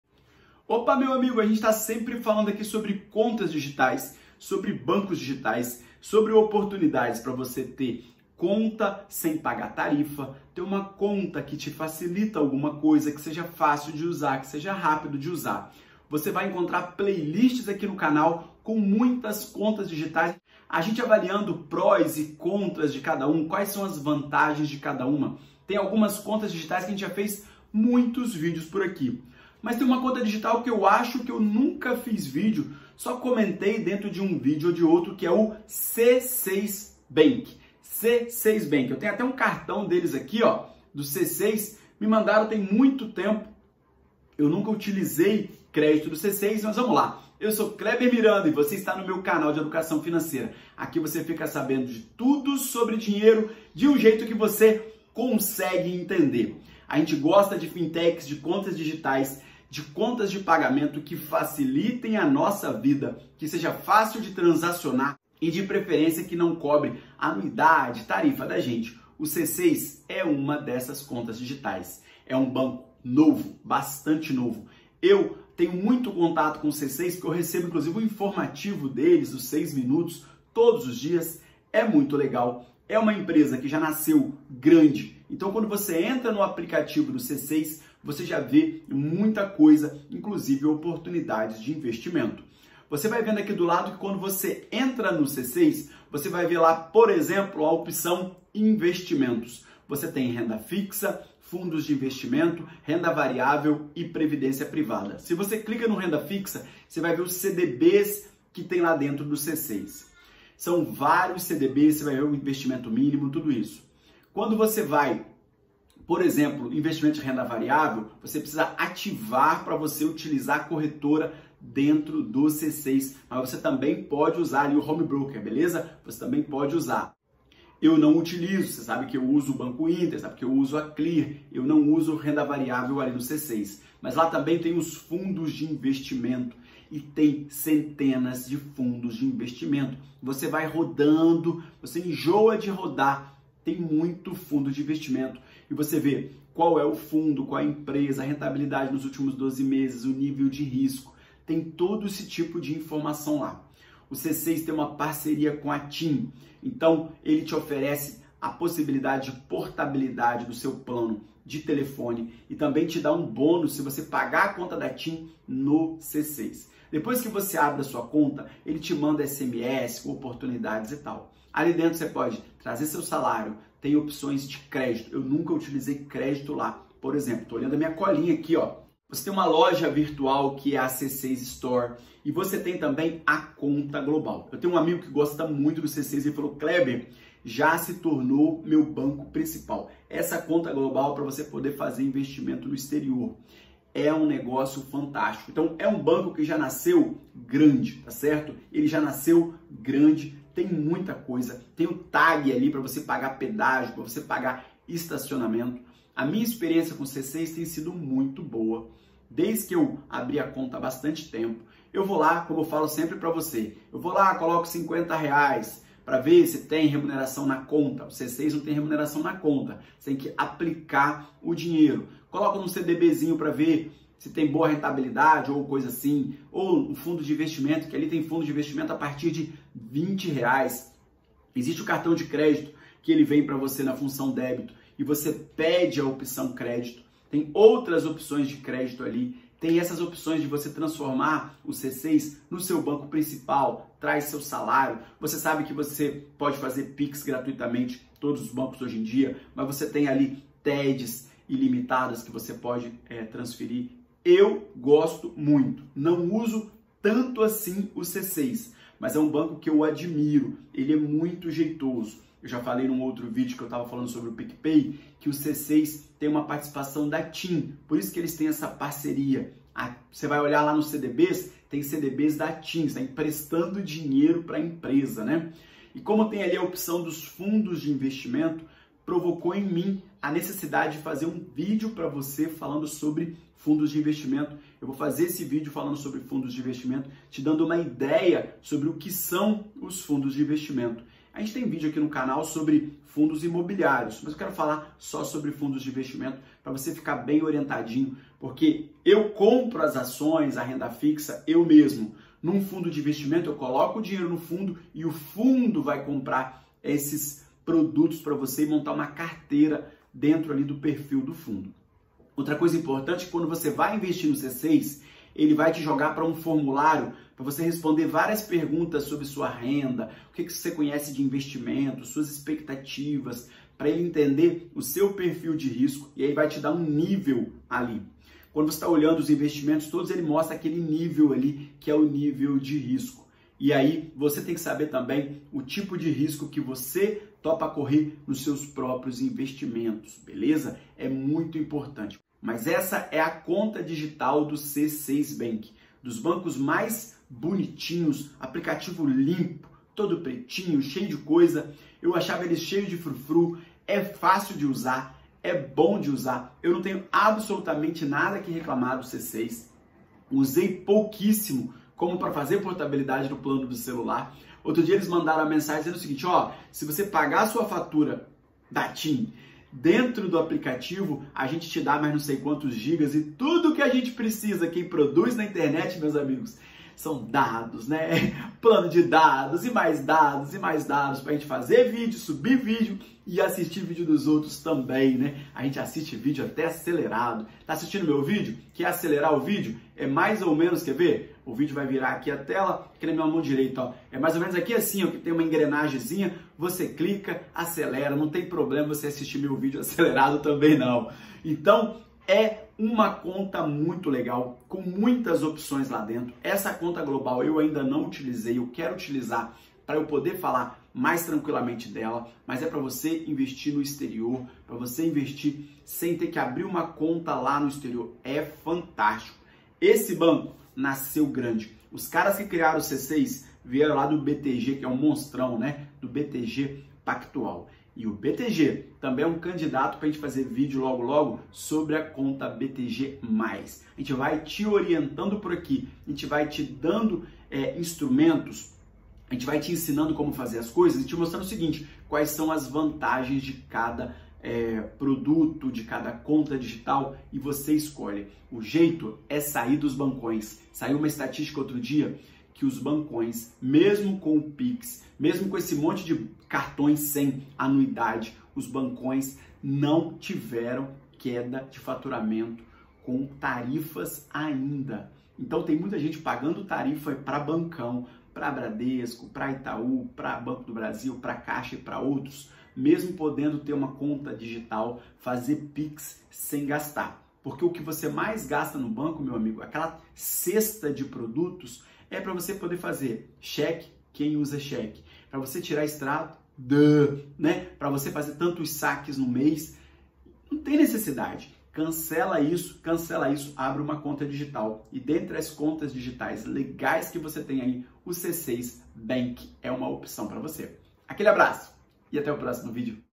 C6 Bank - Conta digital sem tarifas, cartão de crédito sem anuidade - investimentos e conta global
C6 banck conta digital

00:00 - Conta Digital C6

Auxílio Emergencial 2021 - MP1039 na íntegra - Tire suas dúvidas

CUPOM: CLEBER